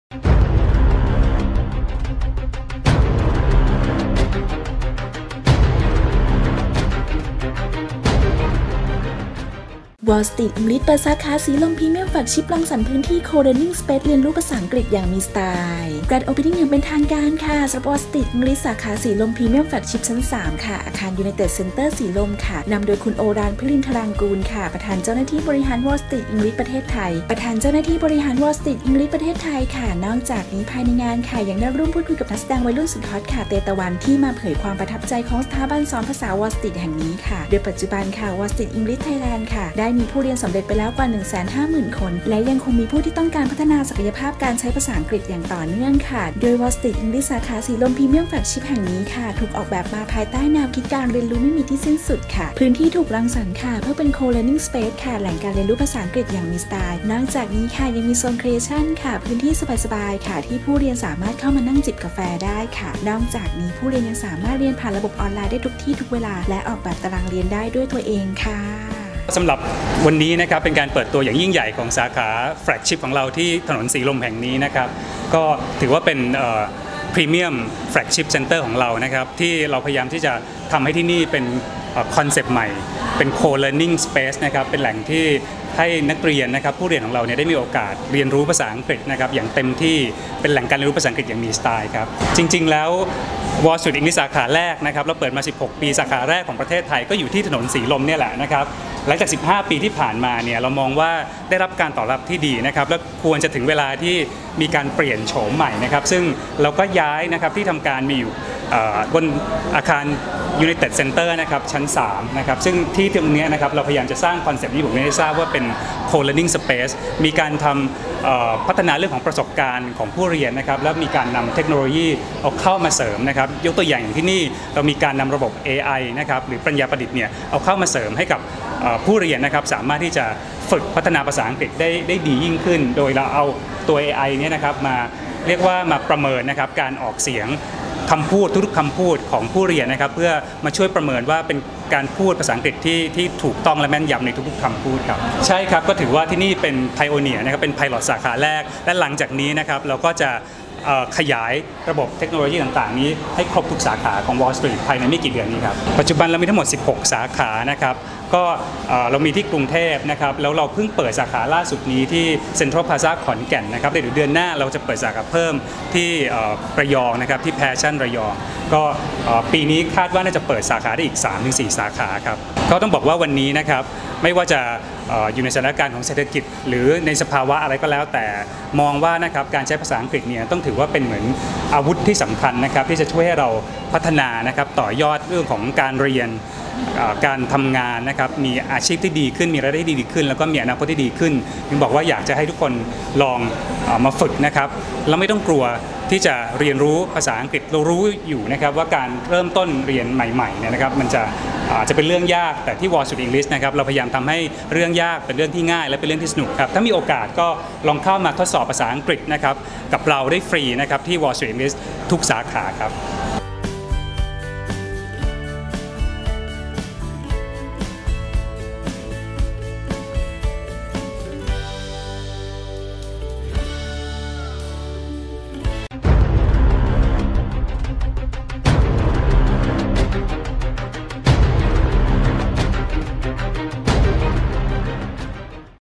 วอลล์สตรีท อิงลิช เปิดสาขาสีลม Premium Flagship รังสรรค์พื้นที่ co learning space เรียนรู้ภาษาอังกฤษ
วอลล์สตรีท อิงลิช เปิดสาขาสีลม Premium Flagship รังสรรค์พื้นที่ co-learning space เรียนรู้ภาษาอังกฤษอย่างมีสไตล์